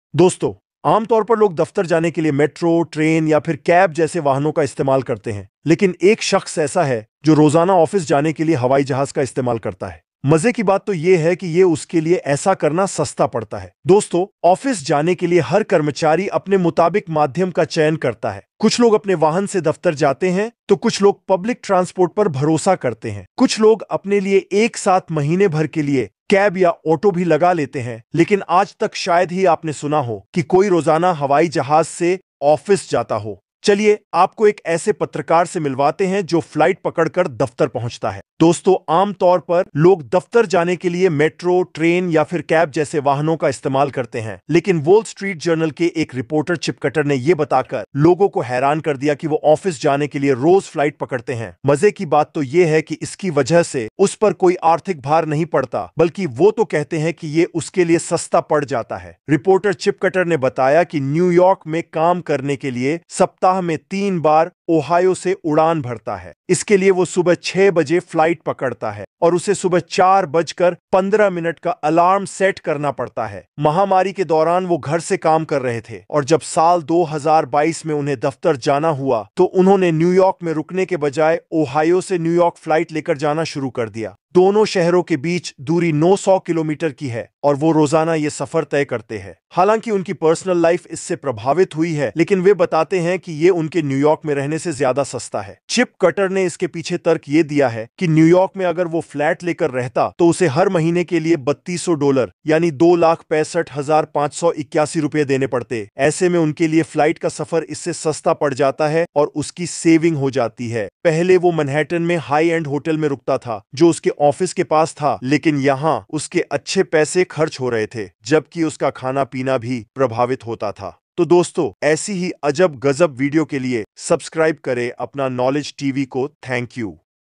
रोज़ हवाई जहाज से ऑफिस जाता है शख्स | Man goes office by flight: A Unique Journey to Work
The Airborne Commuter: A Unique Journey to Work by .'s Workspace

Unconventional Commute
A Journalist's Unique Commute
Chip Cutter's Story
Conclusion